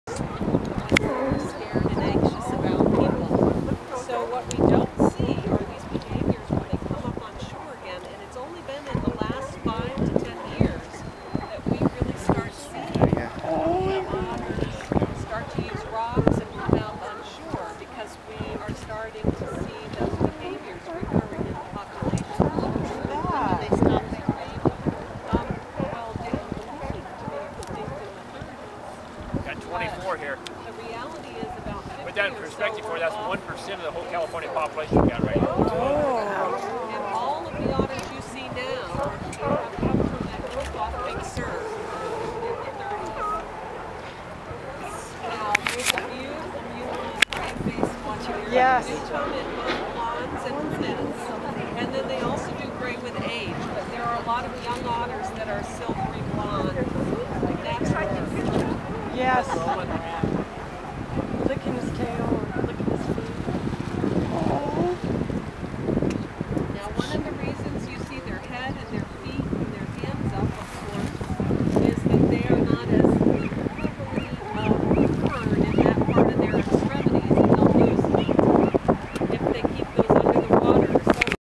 Large raft of sea otters at Moss Landing, CA - April 2011
Short clip of a large raft of sea otters at Moss Landing, CA. Taken on April 9, 2011 from a pontoon boat while on Elkhorn Slough Safari Cruise.